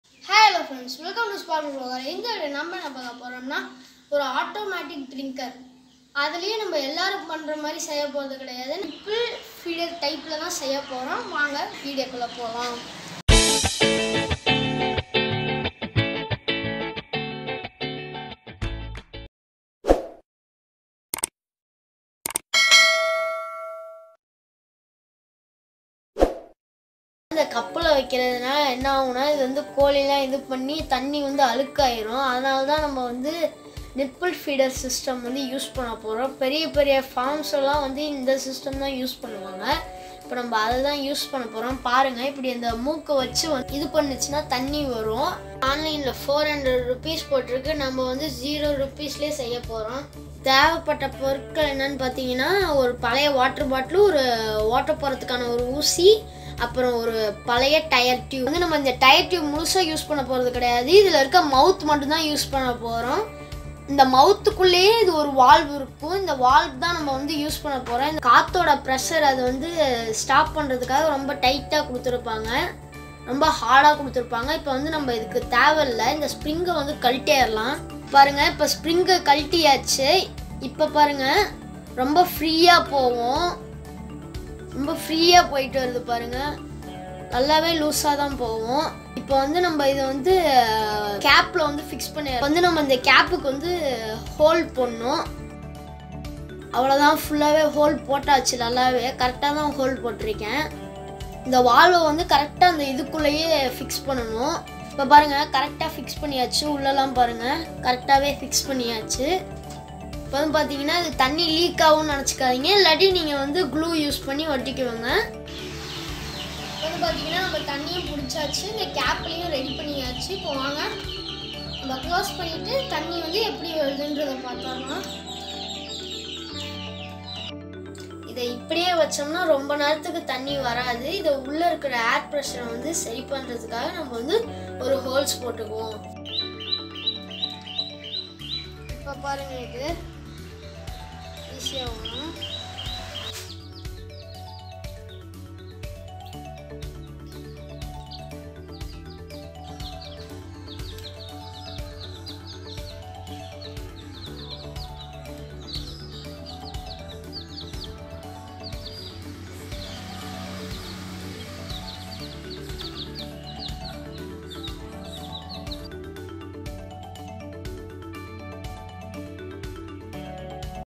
Zero Cost Water Drinker For Birds | DIY | Automatic Water Drinker | Nipple Drinker
This drinker made of waste. zero cost. This drinker suitable  for any bird and rabbit cage. Try it and Enjoy